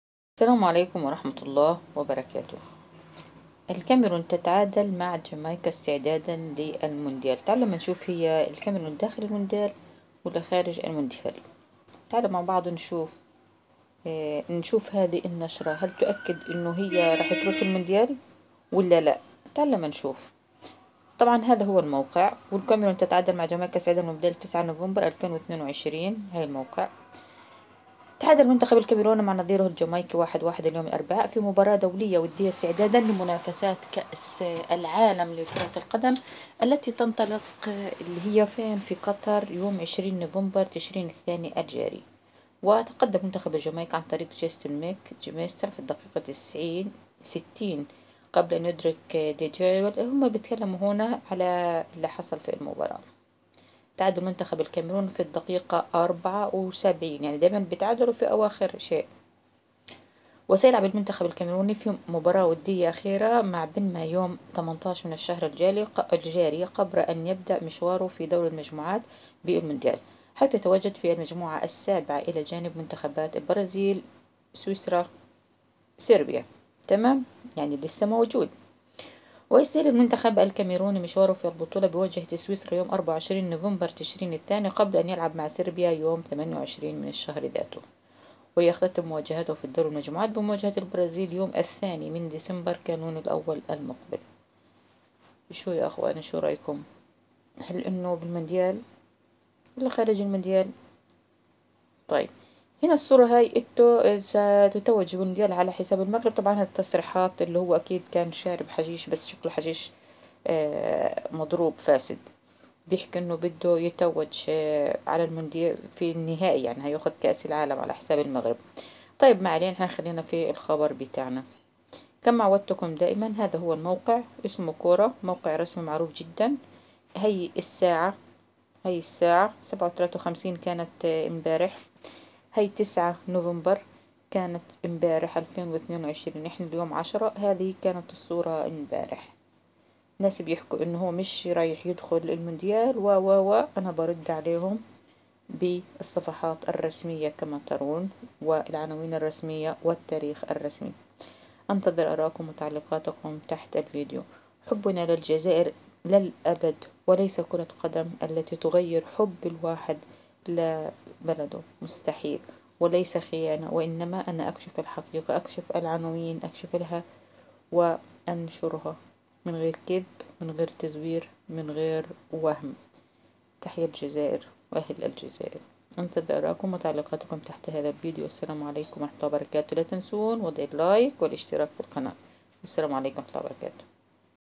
بالموقع الرسمي والتاريخ المنتخب الكاميروني سيذهب للمونديال ام لا؟؟؟؟
يوميات_شاديه_العموري بالموقع الرسمي والتاريخ هل سيذهب المنتخب الكاميروني الى المونديال ام لا؟؟؟؟